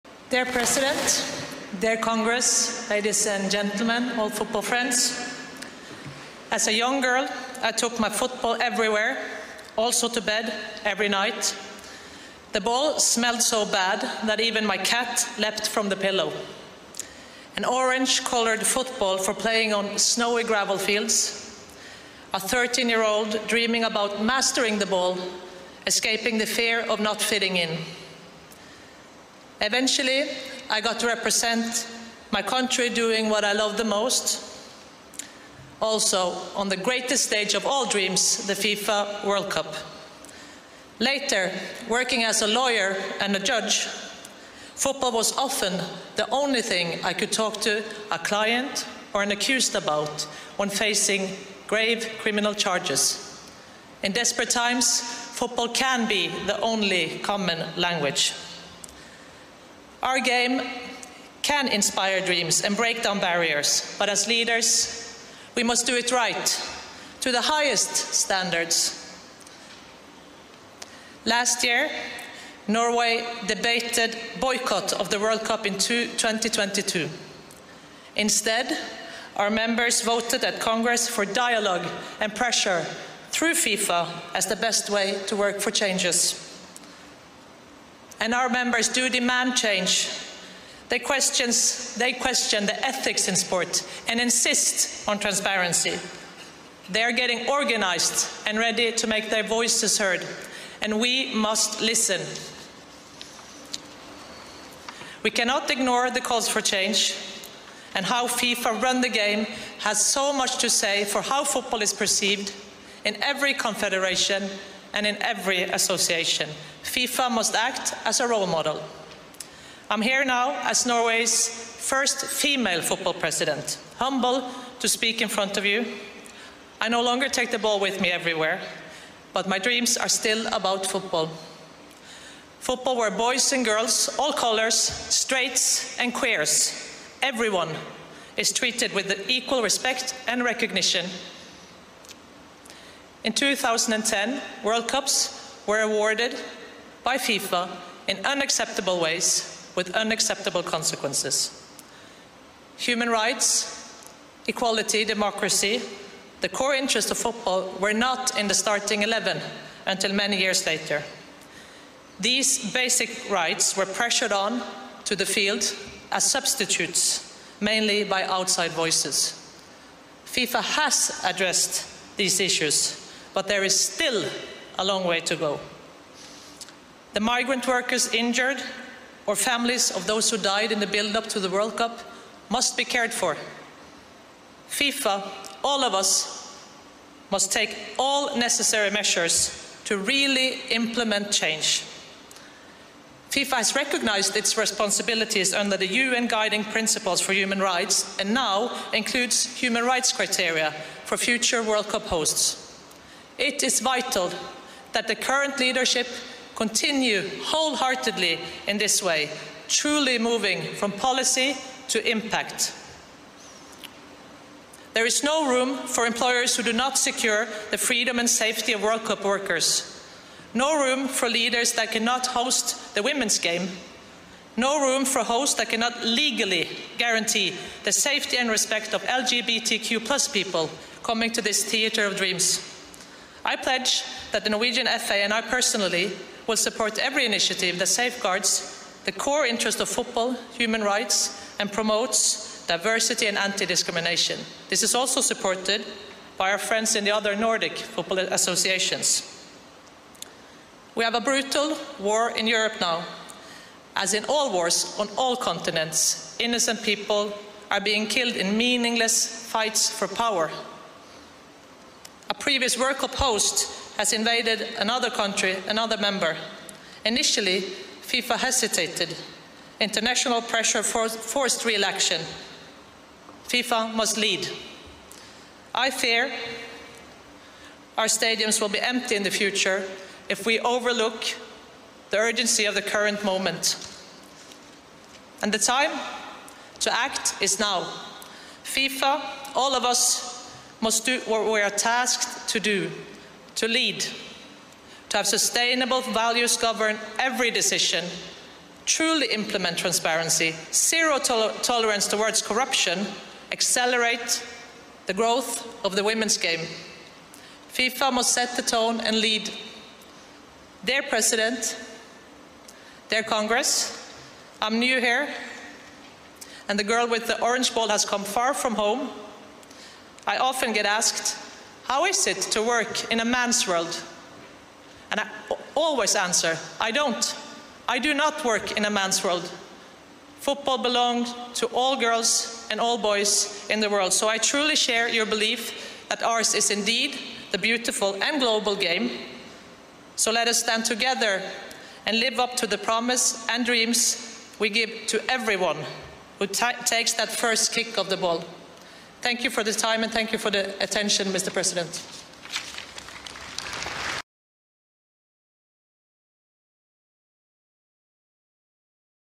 Lise Klaveness FULL SPEECH at FIFA Congress
Lise Klaveness, Norway's first femal FA president, kills FIFA in her speech.